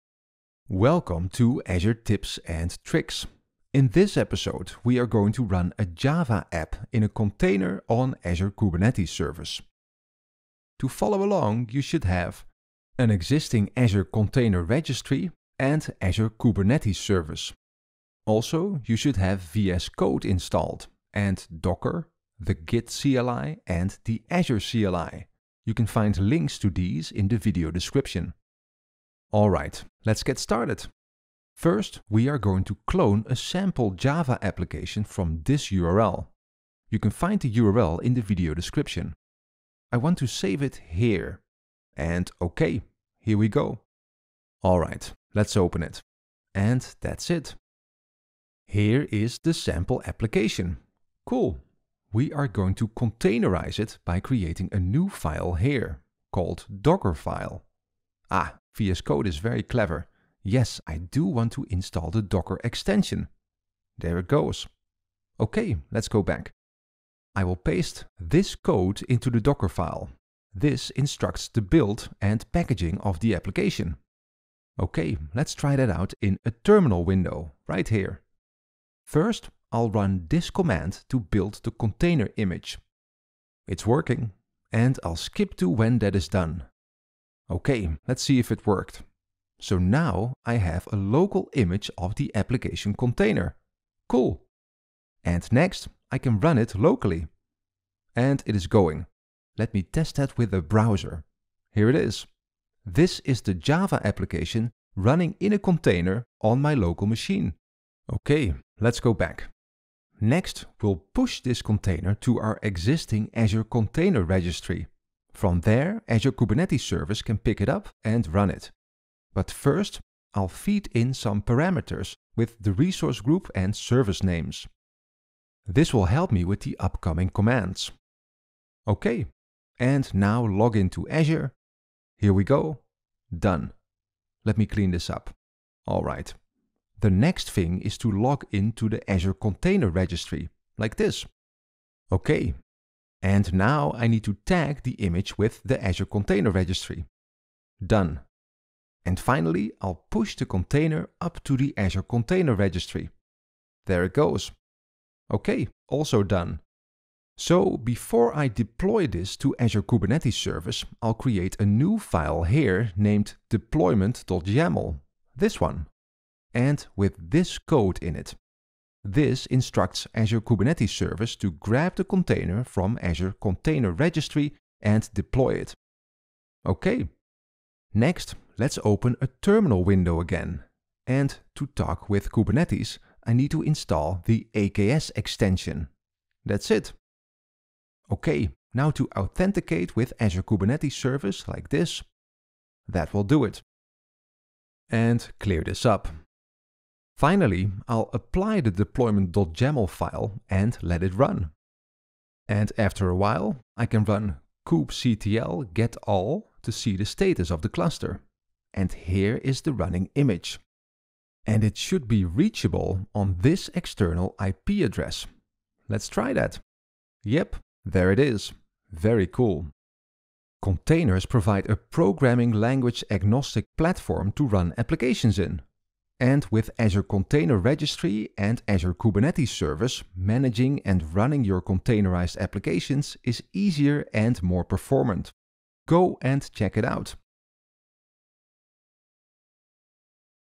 How to containerize and deploy a Java app to Azure | Azure Tips and Tricks
In this edition of Azure Tips and Tricks, learn how to containerize and deploy a Java app to Azure.

Get started with 12 months of free services and $200 USD in credit. Create your free account today with Microsoft Azure